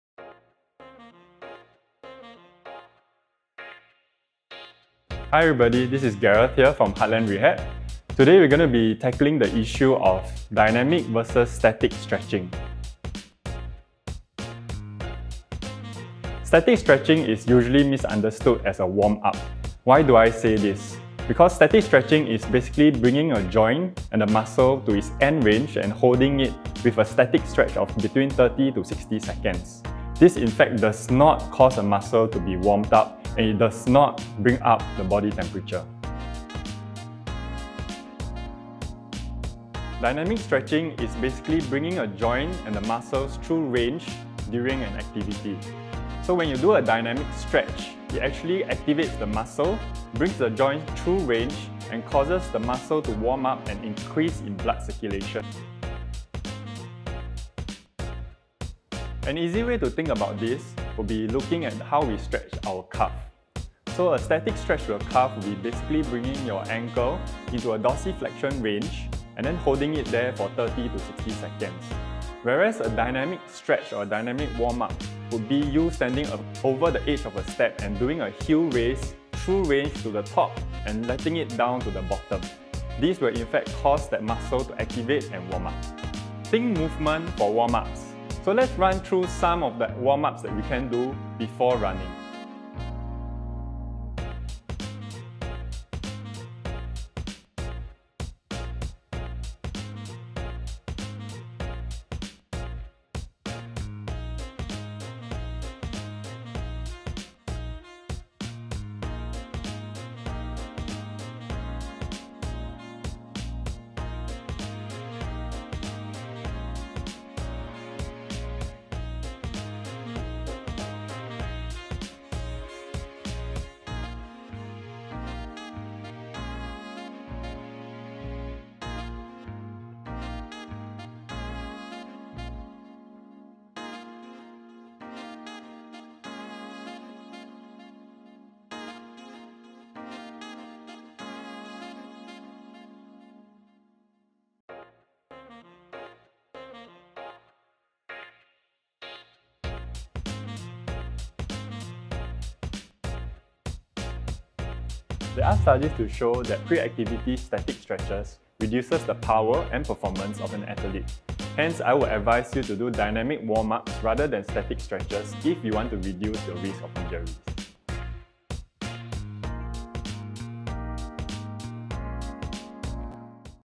Just For The Runners: Dynamic VS Static Stretching
🎽🏃Just For The Runners: Dy🎽🏃Just For The Runners: Dynamic VS Static Stretching

Do you stretch before running? If you do, what type of stretch do you usually do?

There are 2 common stretches that we do: dynamic and static

They are very different from each other, and yes - there is one that's better than the other when it comes to warming up before your runs 👀!

🤔 Can you guess which one is the better one?

P.S. Watch till the end to learn 6 warmup stretches that Gareth recommends! Save this for your next run too.
@heartlandrehabsgnamic VS Static Stretching